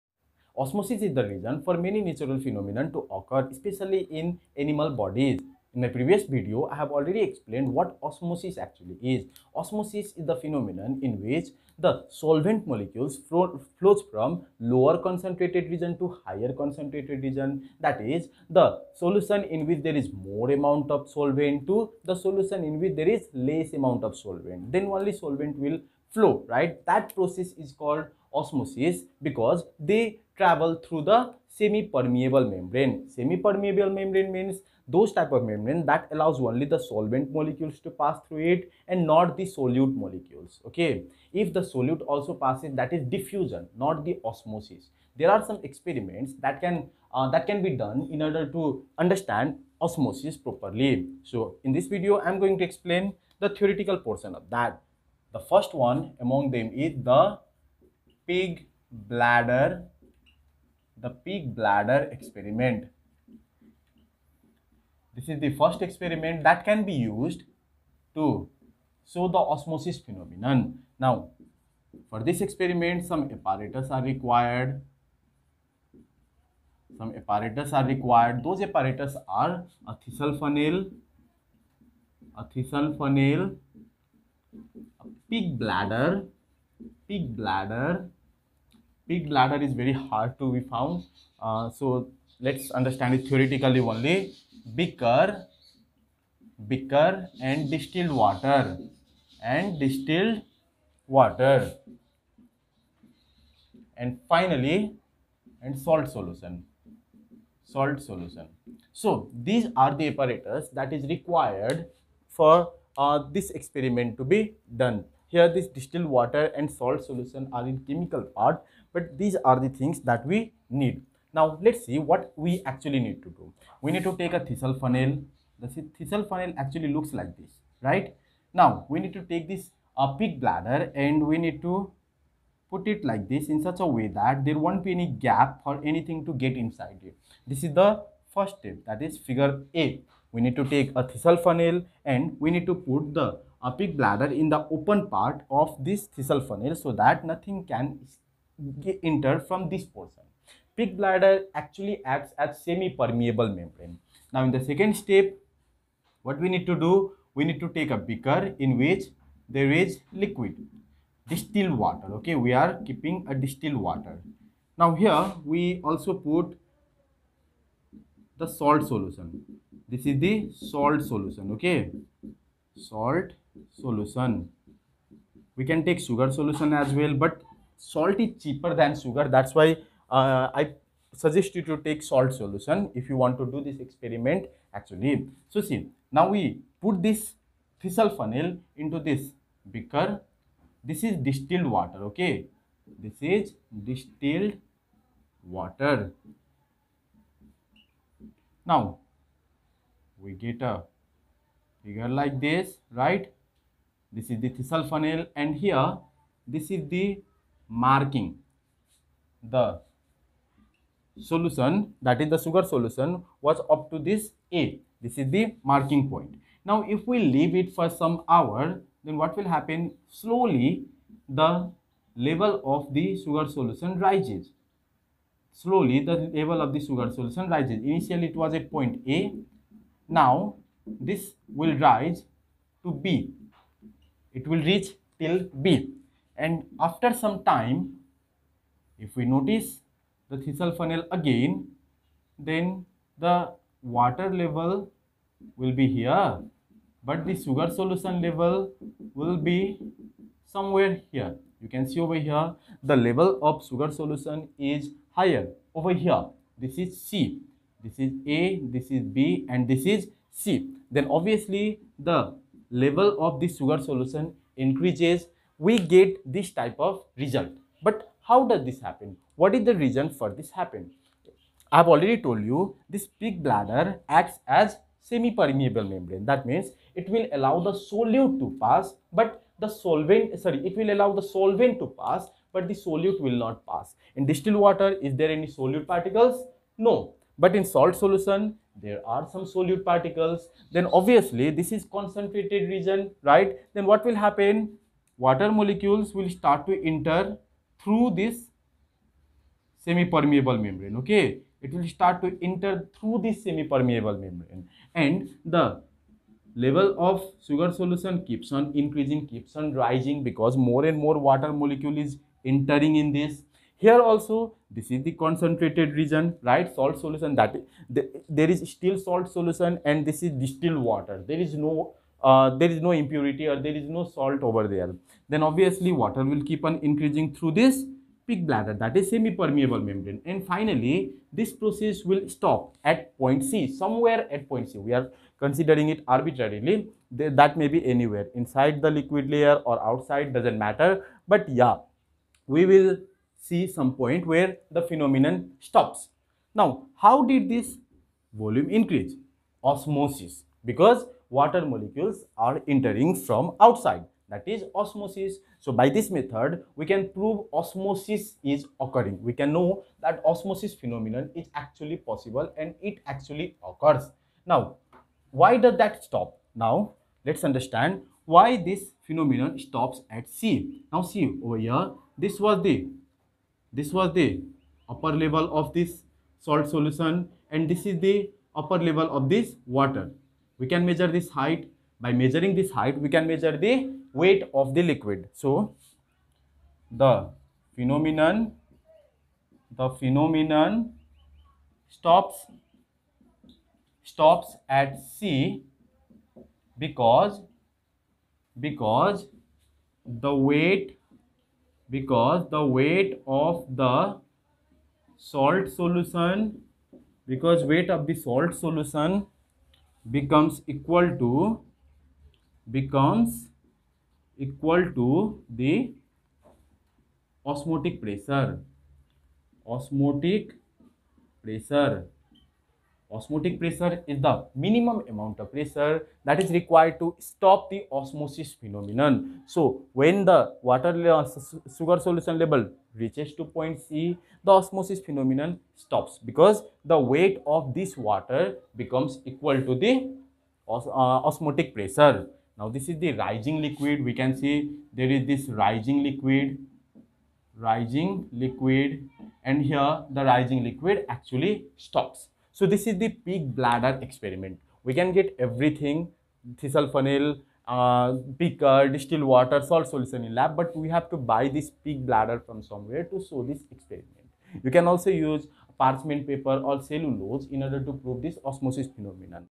The pig bladder experiment for Osmosis (Theory).. Colligative properties..
To show osmosis phenomenon, there are few experiments that can be done in laboratory. Among them, a very important experiment is 'The Pig Bladder Experiment'. In pig bladder experiment we can see the osmosis phenomenon by using a thistle funnel and a pig bladder (semipermeable membrane).
In this video I have explained about the theory of the pig bladder experiment. By watching this video you will get all the knowledge about the experiment and you can perform this experiment in the lab too.

Presented by Govinda Thakur (Samir Sharma)
MSc Chemistry
Tribhuvan University